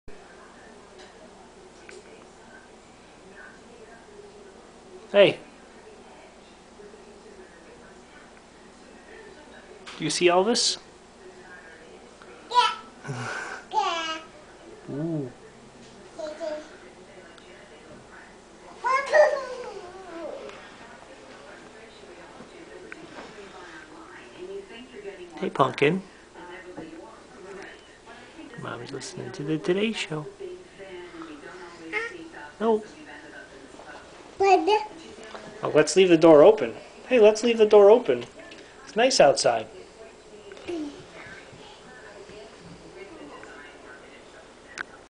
Watching Elvis
Watching Elvis from the door.  (Elvis is the cat, btw)